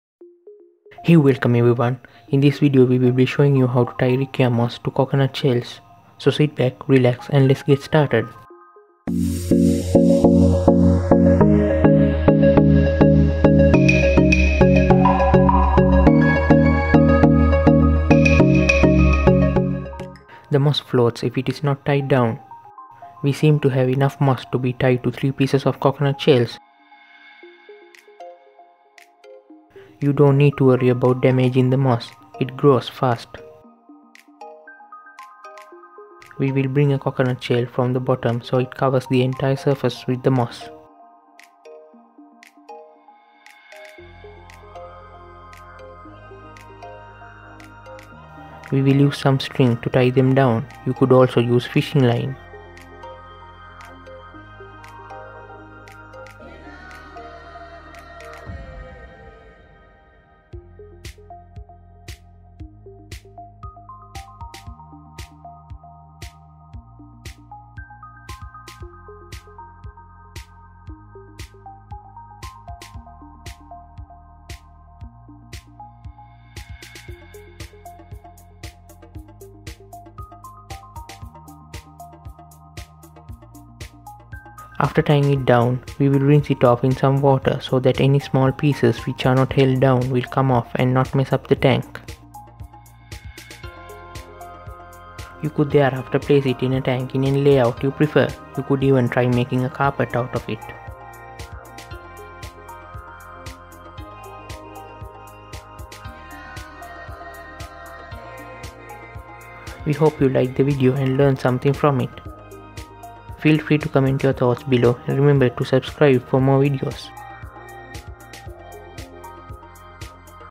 Tying Riccia Fluitans Aquatic Moss to Coconut Shell (How to DIY)
Tying Riccia Fluitans Aquatic Moss to Coconut Shell

“Hey welcome everyone … in this video we will be showing you how to tie riccia moss to coconut shells. Sit back, relax and let’s get started”
“The moss floats if it is not tied down. We seem to have enough moss to be tied to three pieces of coconut shells, so I will separate them out evenly. You don’t need to worry about damaging the moss, it grows fast. We will bring a coconut shell from the bottom so it covers the entire surface with the moss. We will use some string to tie them down, you could also you fishing line. After tying it down, we will rinse it off in some water so that any small pieces which are not held down will come off and not mess up the tank. You could thereafter place it in the tank in any layout you prefer, you could even try making a carpet out of it”
“We hope you liked the video and learned something from it.